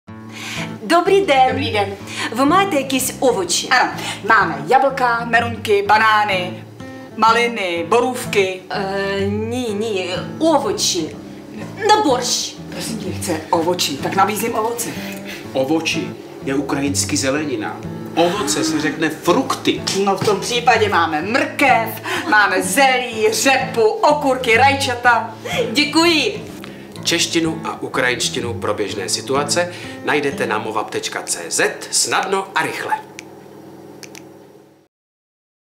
Ovoce – Movapp.cz
Вивчіть основи чеської та української мови для повсякденного життя.
Learn the basics of Czech and Ukrainian for everyday life situations.

Hrají:
Inna Bovkun-Mishakina – Kyjev

Herci Západočeského Divadla v Chebu –
David Beneš a Vladimíra Vítová